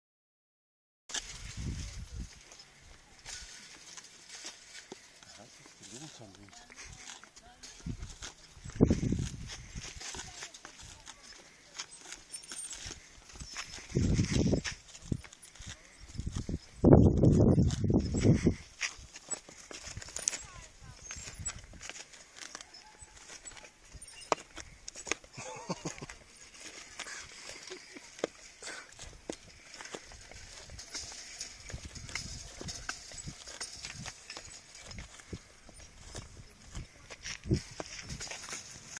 Straußenfarm auf Mallorca
Die Straußenfarm Artestruz bei Campos / Mallorca 2011. Für Kinder und Erwachsene ein tolles Erlebniss.